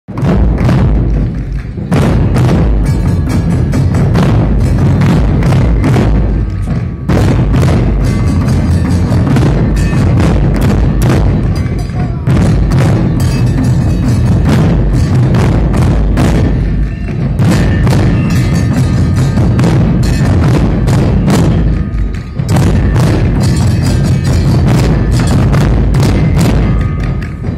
Folión As Maceiras, en Vilamartín
Folión As Maceiras, en el encuentro de folións de Vilamartín de Valdeorras.